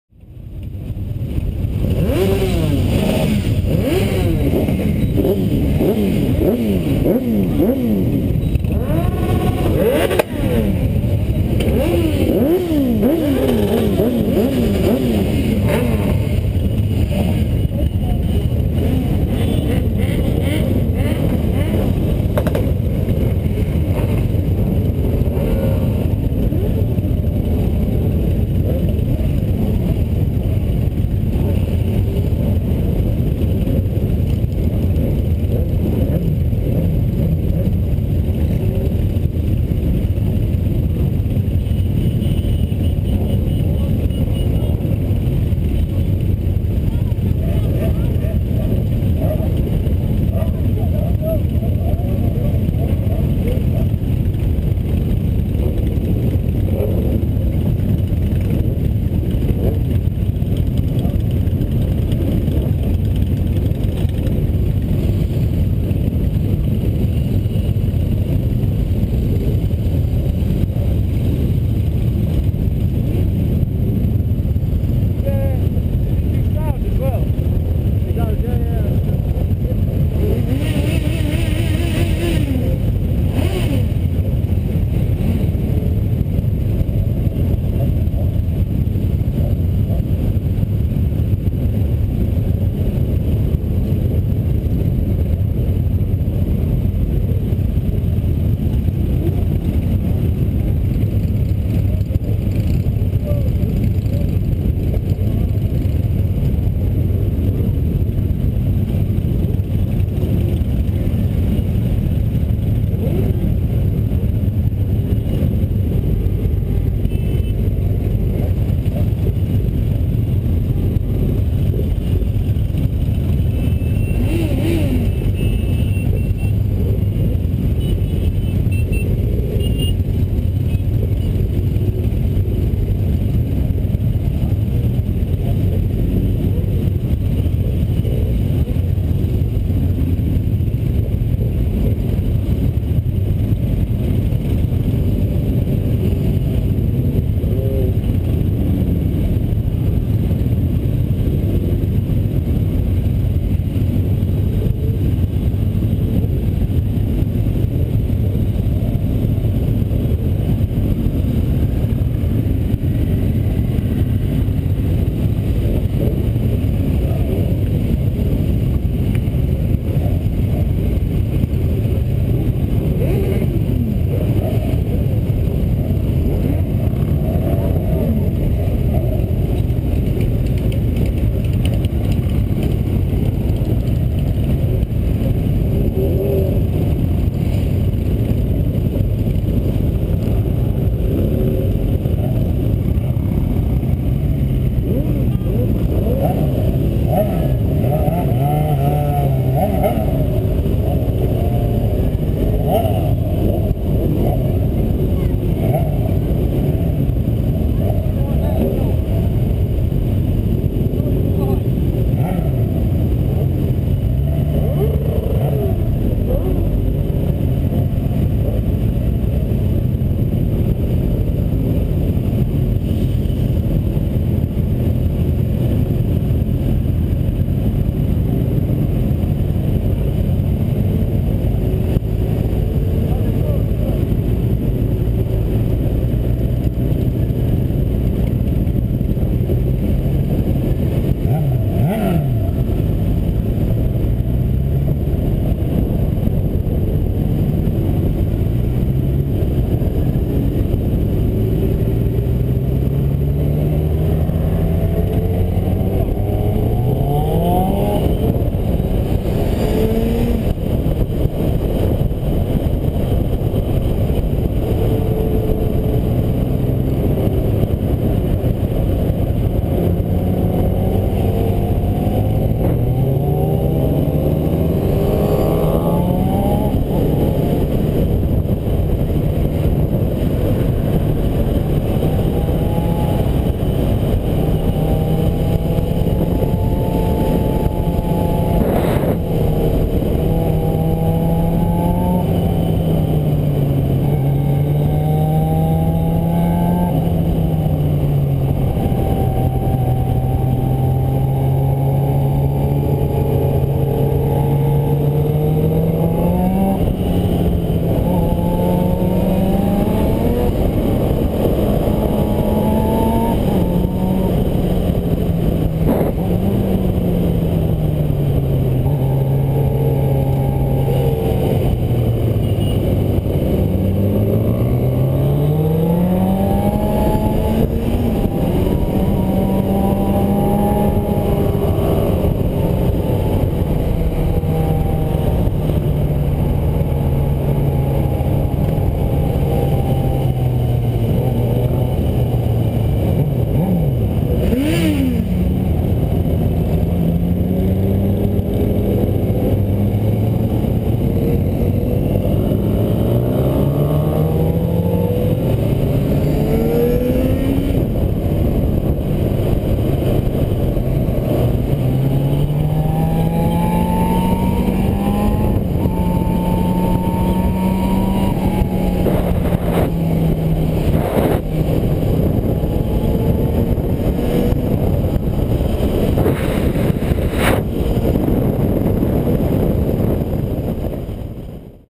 Essex Air Ambulance 2012 Motorcycle Run - Departure
Departure from Dunton Technical Centre for the Motorcycle Run 2012